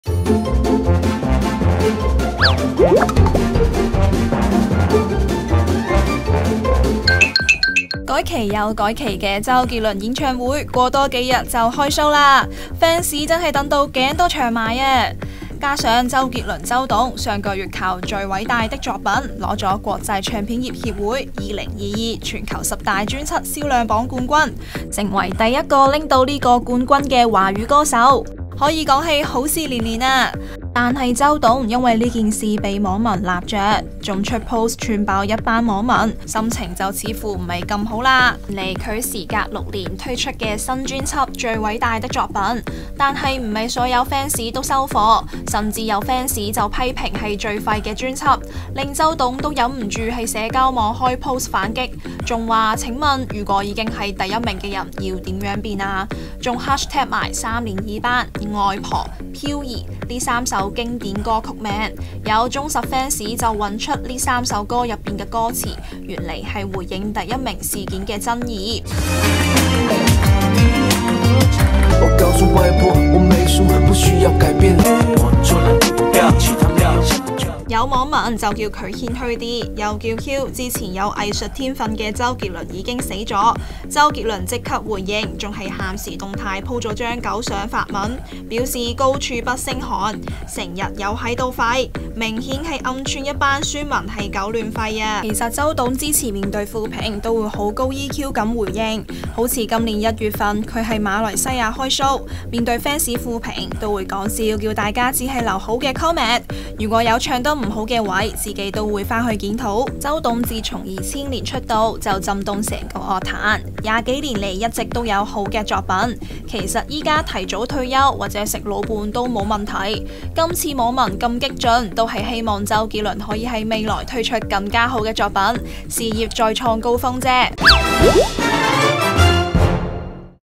自封第一被插 周杰倫嬲爆寸黑粉「狗吠」
周杰倫爆寸黑粉「狗吠」，咩事搞到周董咁扯火！？